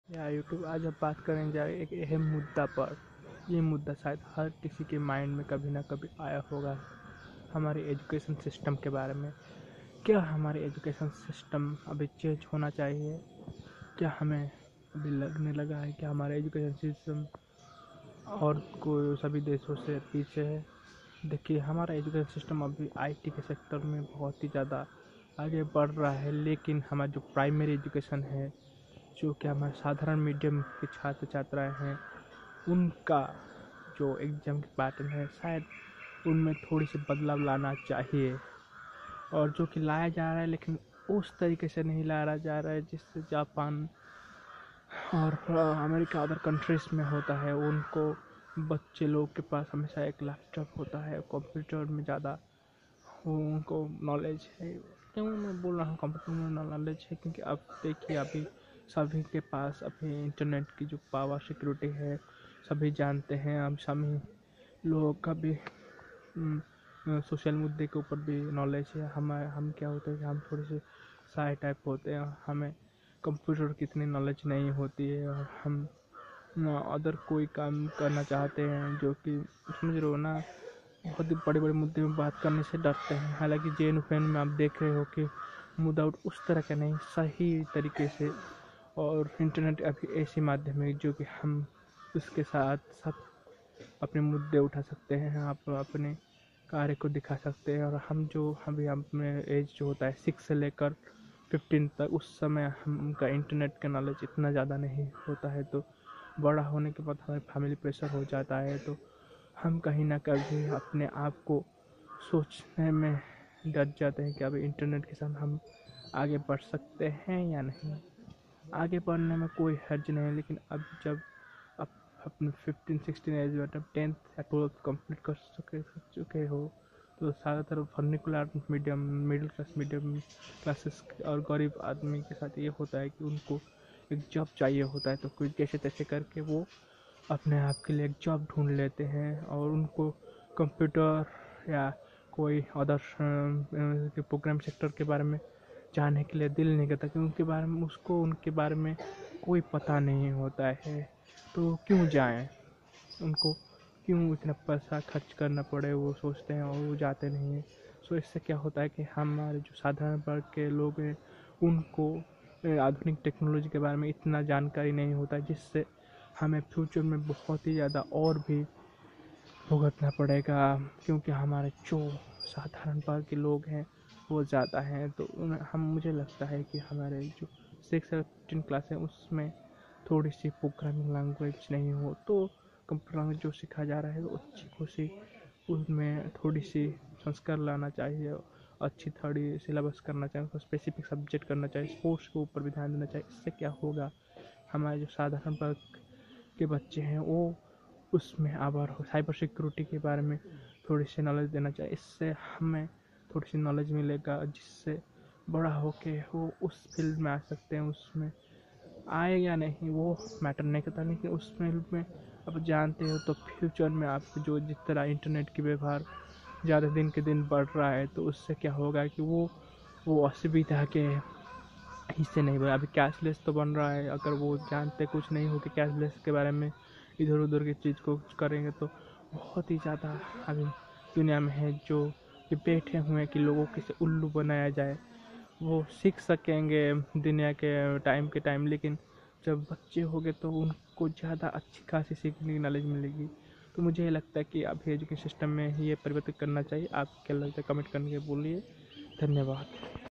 India internet education
This video yeah
What OUR education system and how help cyber education special government school and hindi and other regional language school